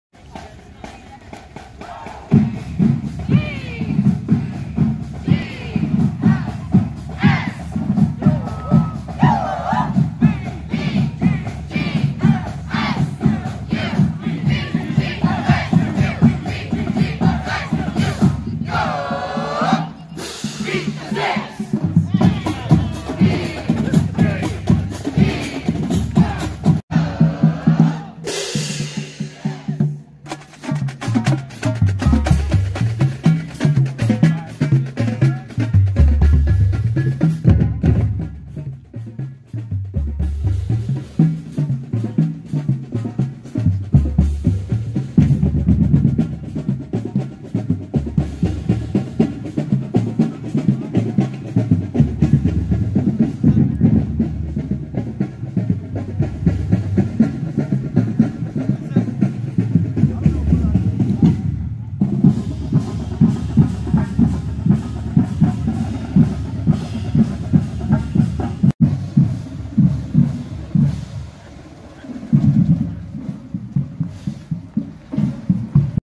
Falcon Marching Band 2021 Akron Game Parade to Stadium
The Falcon Marching Band from Bowling Green, Ohio, stepping off from Bowling Green State University's Moore Musical Arts Center in Bowling Green, Ohio, and parading on Ridge Street in Bowling Green, Ohio, to the MAC East football game between the Bowling Green Falcons of Bowling Green, Ohio, and the Akron Zips of Akron, Ohio, played at Bowling Green State University's Doyt L. Perry Stadium in Bowling Green, Ohio, on Saturday, September 9th, 2021.